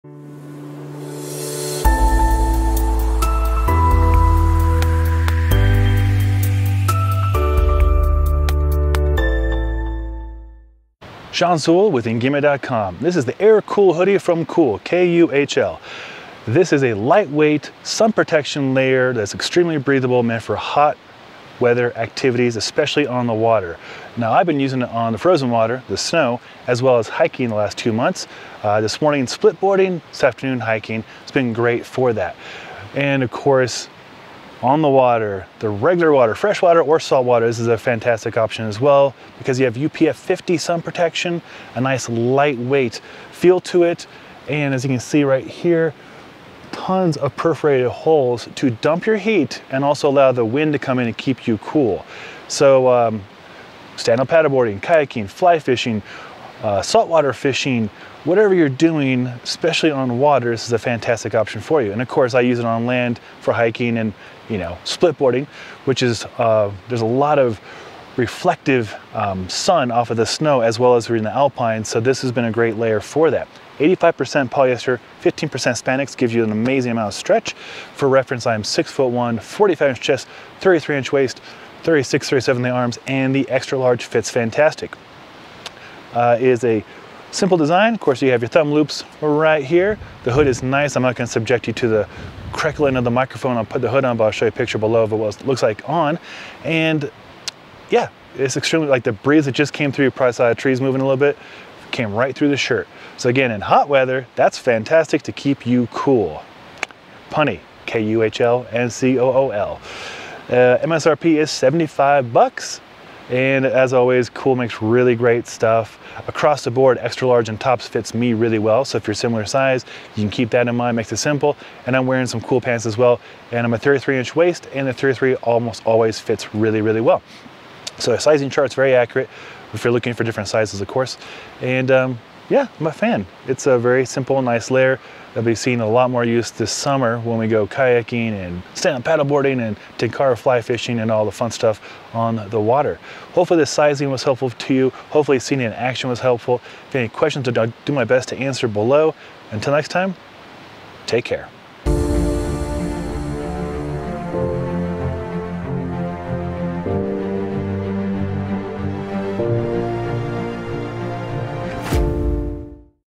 Kuhl AirKuhl Hoody Review - UPF50 Hoody for Hot Weather Activities
The Kuhl AirKuhl Hoody is a well-fitting, good-looking long sleeve shirt that is great for hot-weather activities.  Fishing, hiking, SUP, rafting, and anything in the sun and heat!